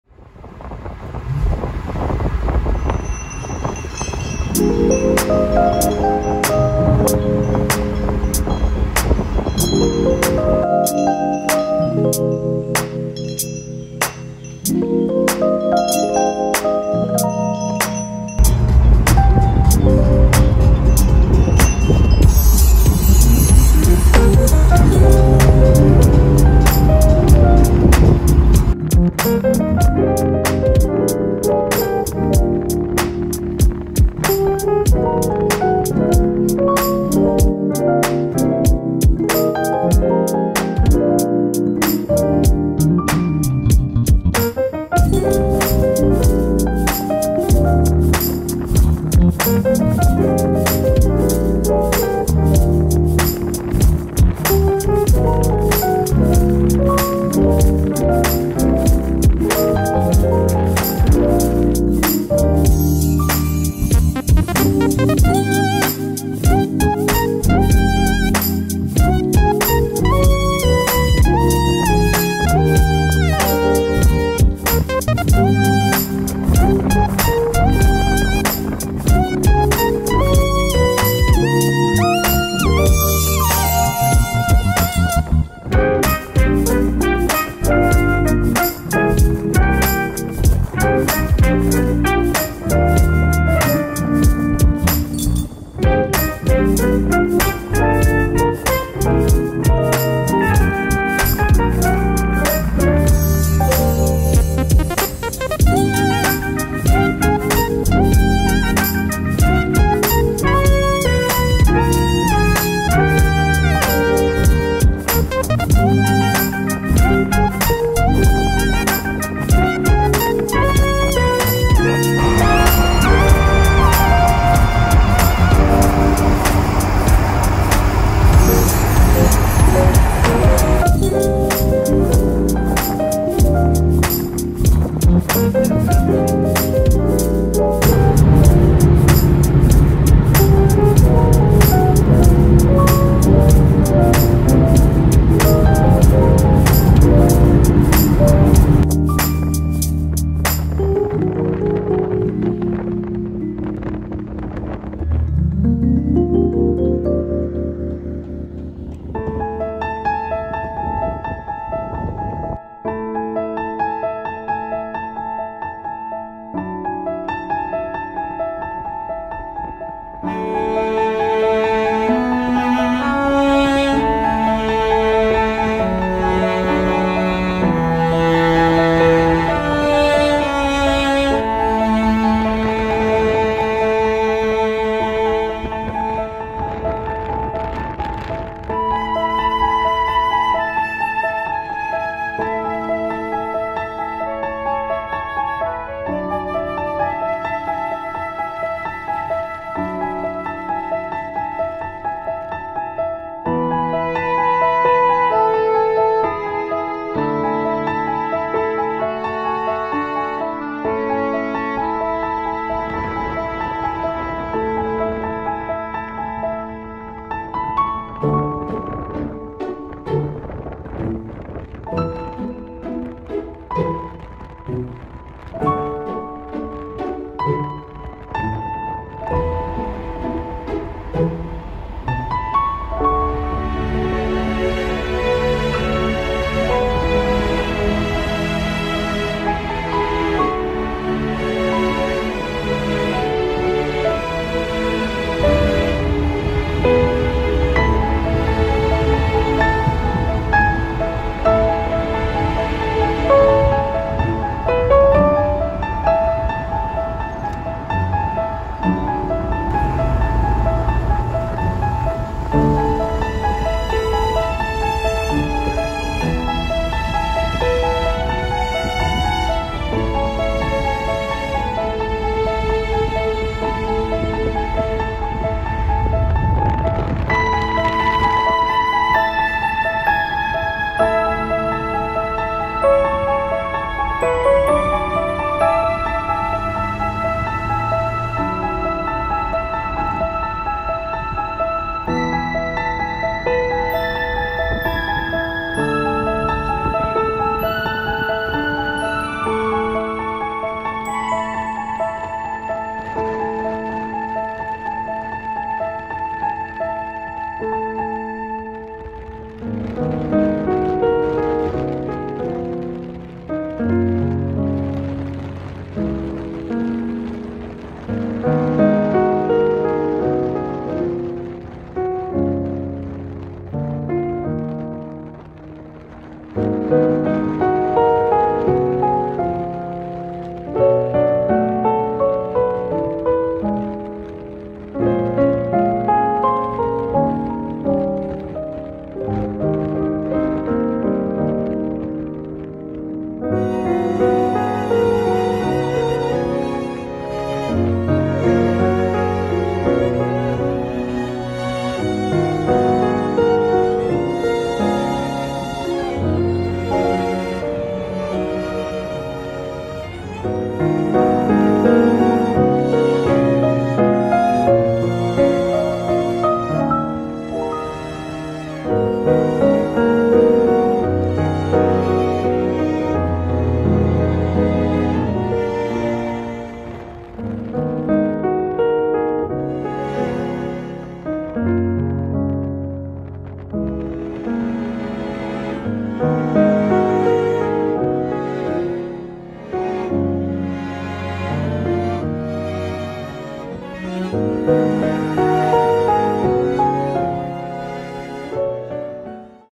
오픈카 타고 독일 드라이브 여행! Germany convertible ride drive trip
그동안 여행했던 몇주간의 일상을 한 영상에 담아보았습니다. 코로나로 인하여 전세계가 여행이 마비되었는데, 이 영상이 여러분께서 함께 독일 드라이브 여행하는듯한 감성을 불러일으키길 바라며...

피아니스트 유경식
카톡ID: ksyu721

👍유튜브 좋은 영상들만 바로 골라보기 안내!
👍환상적인 영상강좌 후기 보기!